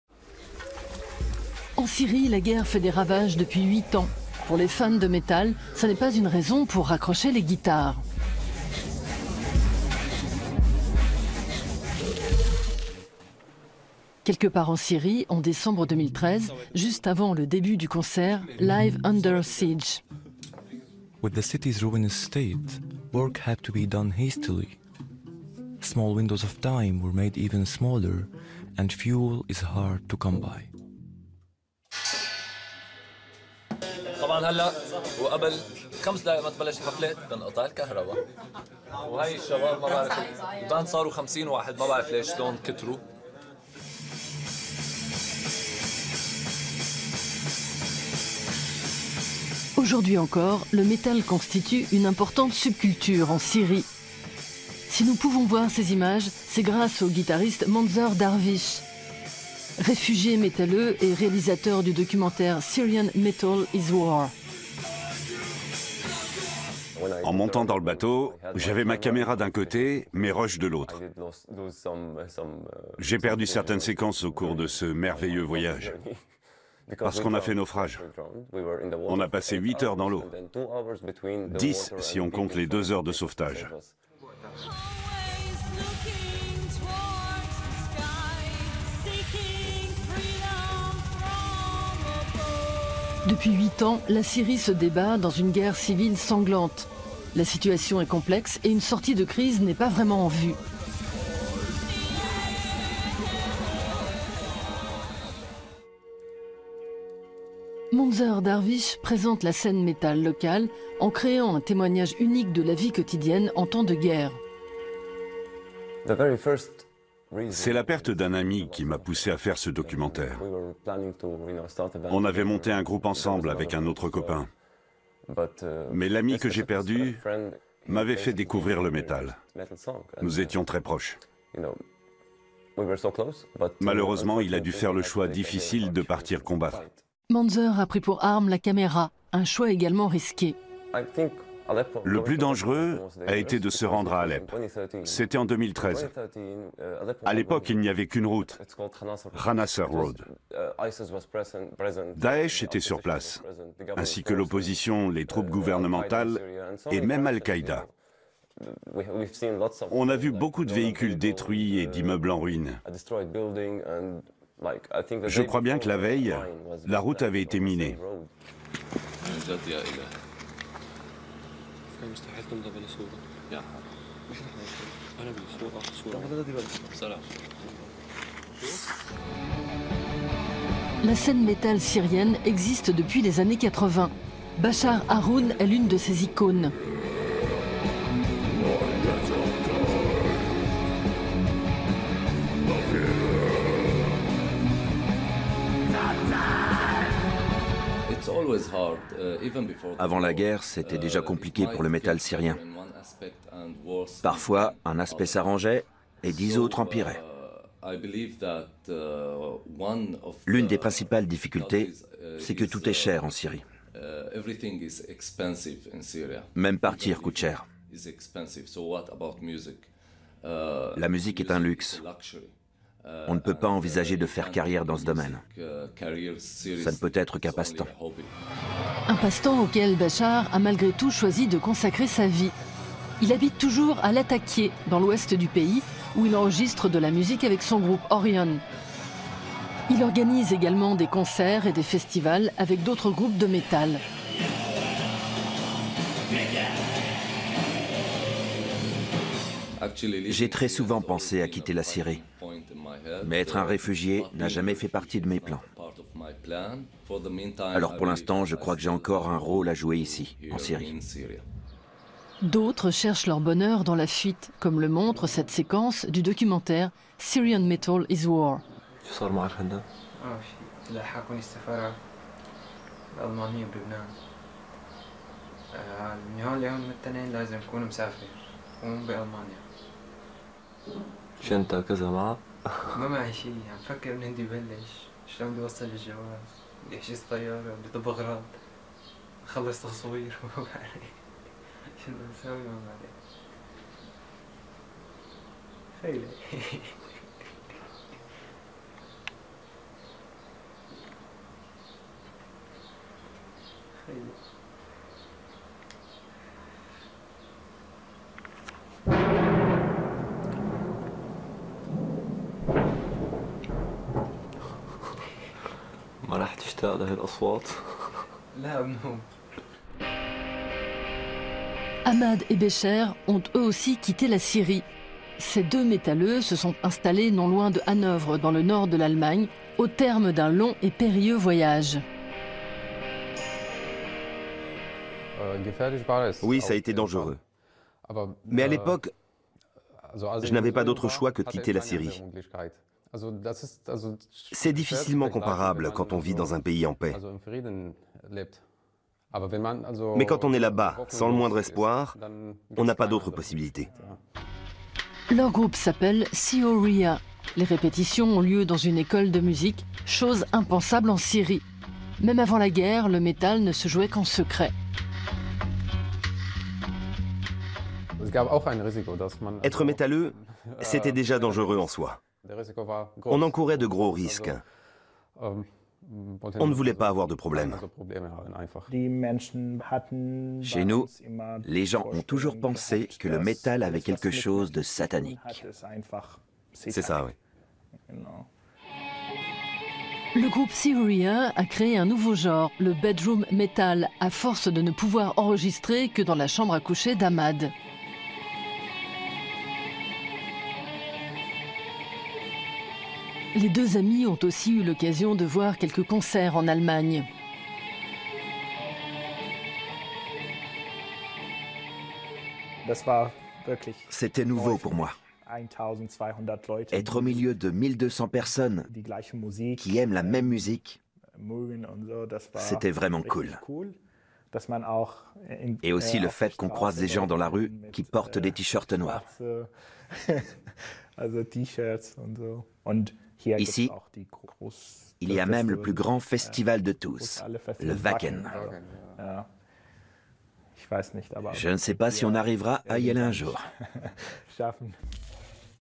Syrian Metal | TRACKS - ARTE
Une subculture au milieu des décombres : malgré huit ans de guerre civile, le heavy metal est encore bien vivant en Syrie. Gros plan sur une scène musicale qui parvient encore à s’épanouir malgré les horreurs – au pays et depuis les terres d’exil.